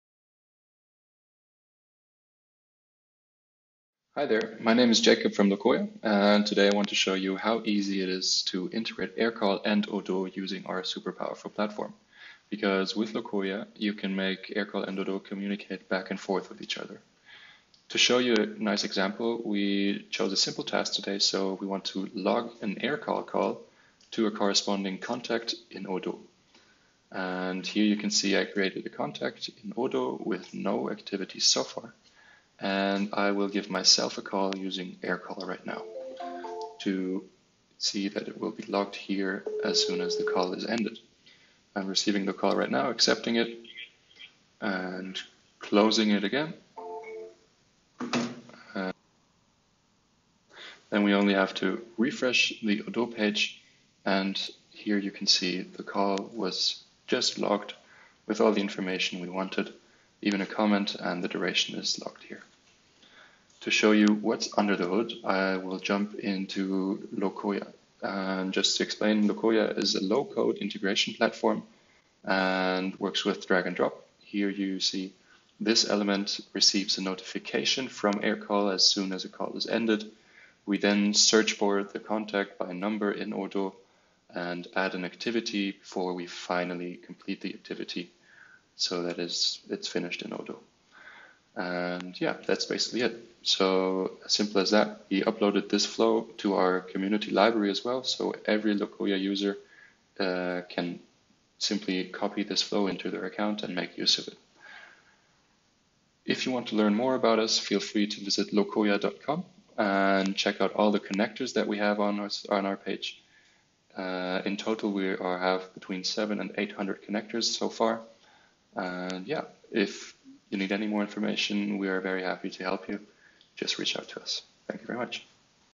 Connect Aircall with Odoo to log calls in the Odoo CRM - Integration made easy
AirCall to Odoo integration

Did you know? You can easily integrate Aircall into your Odoo account.

Many clients asked us for this integration so today we want to share with you how easy it actually is to integrate those systems.

All you have to do is copy the template from our community library and you can start syncing your calls into your Odoo CRM.